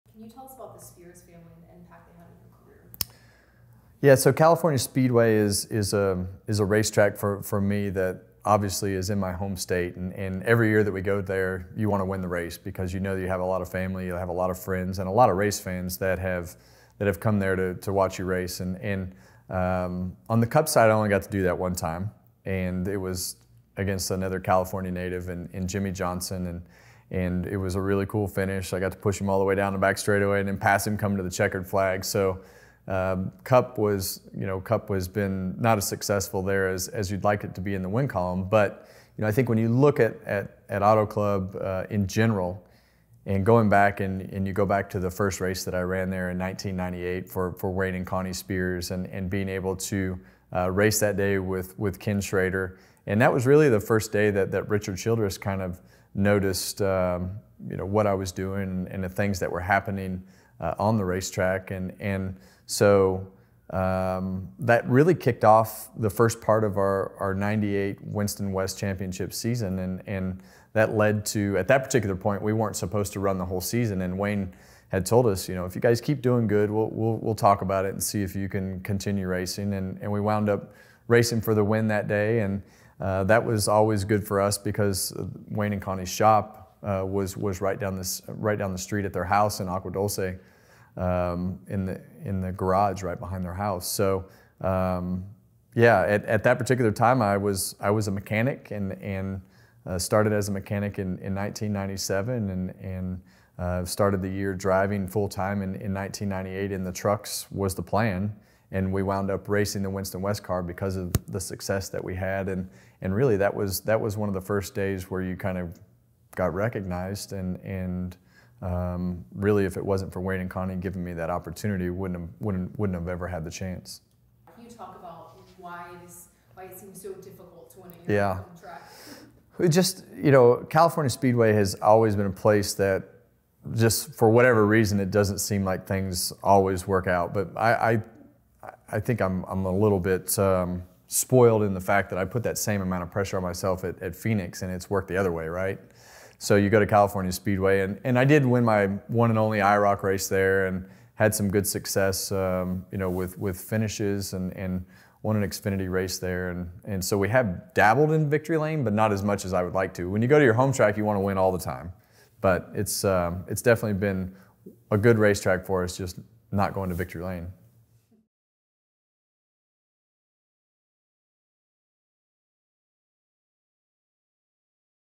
Kevin Harvick Track Stories: Fontana | 4EVER | Stewart-Haas Racing
A native Californian, Kevin Harvick has always enjoyed going to his home track, Auto Club Speedway. Hear from The Closer himself on what the track has meant to him, his success at the 2-mile oval, and his favorite memories of racing at home.